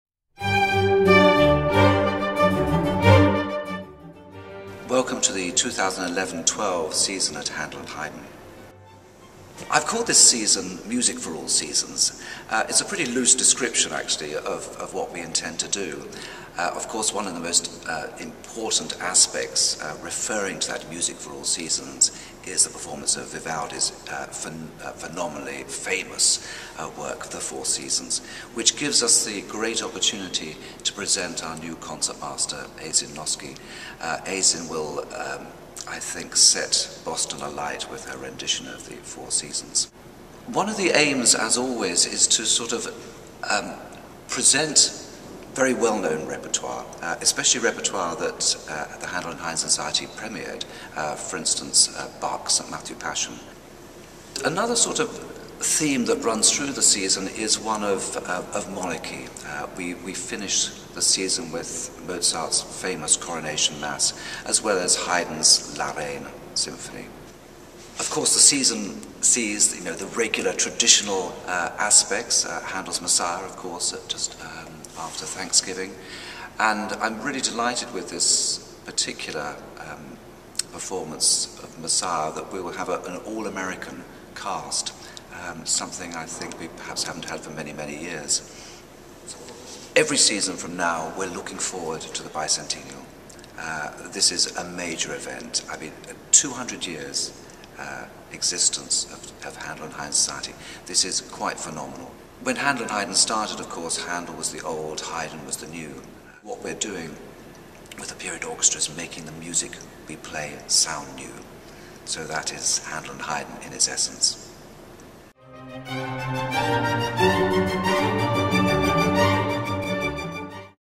Welcome to the 2011-2012 Season
Artistic Director Harry Christophers welcomes you to the 197th Season at Handel and Haydn.